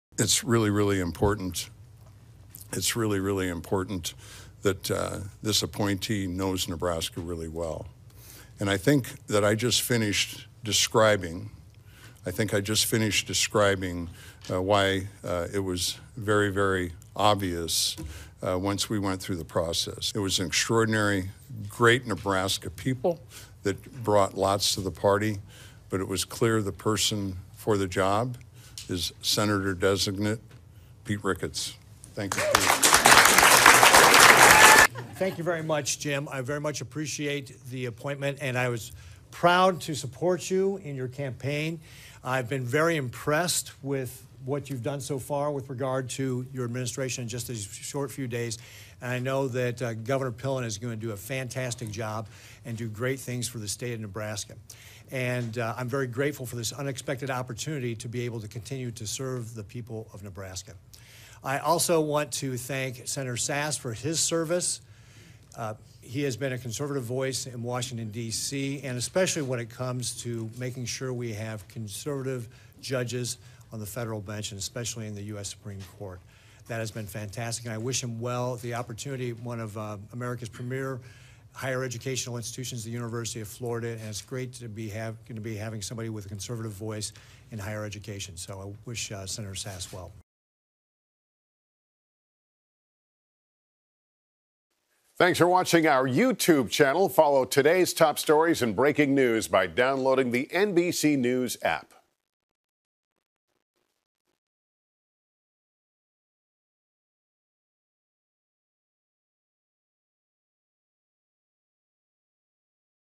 Nebraska governor appoints former Gov. Ricketts to replace Sen. Sasse
Nebraska Gov. Jim Pillen announced that he will be appointing former Gov. Pete Ricketts to fill Sen. Ben Sasse's seat after he resigned to serve as the president of the University of Florida.